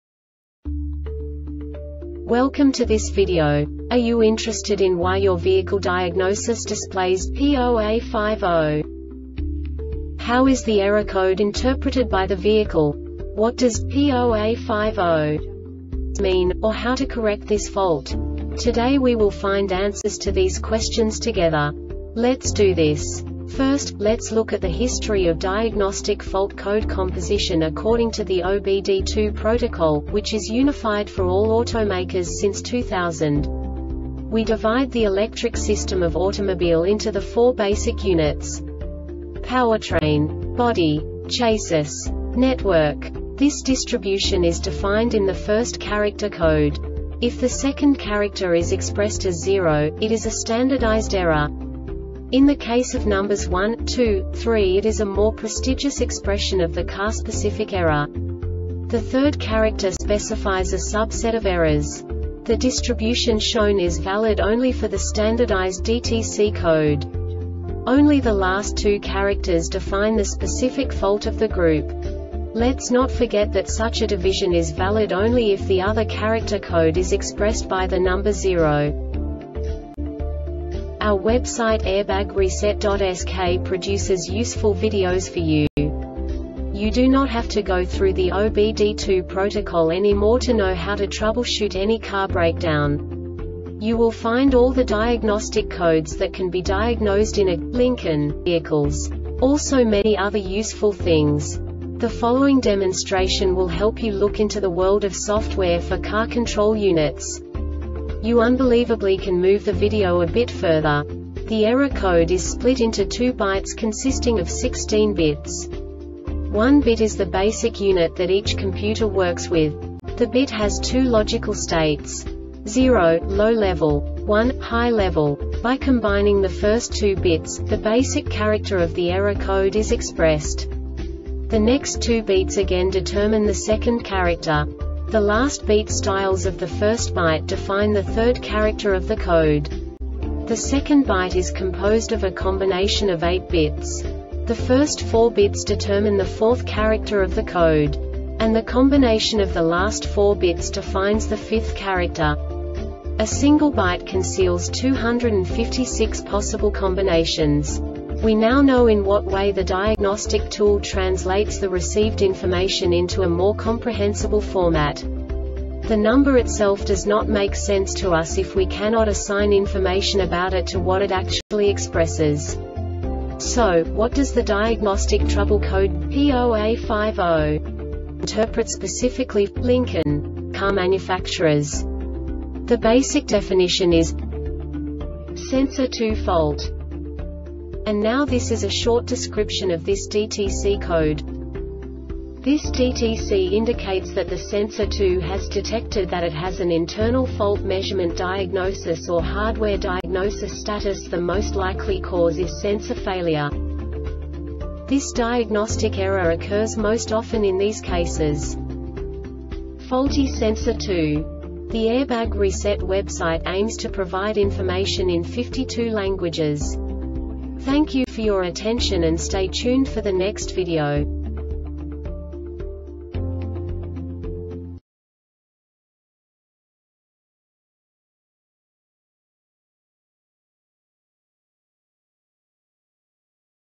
DTC Lincoln P0A50 Short Explanation
Contents:
0:21 Basic DTC analysis according to OBD2 protocol standard.
1:48 Insight into programming
2:58 A diagnostically understandable description of the Diagnostic Trouble Code
3:05 Basic error definition

Make:
Lincoln
Code:
P0A50
Definition:
Sensor 2 Fault
Description:
This DTC indicates that the Sensor 2 has detected that it has an internal fault (measurement diagnosis or hardware diagnosis status).The most likely cause is sensor failure.
Cause:
 Faulty Sensor 2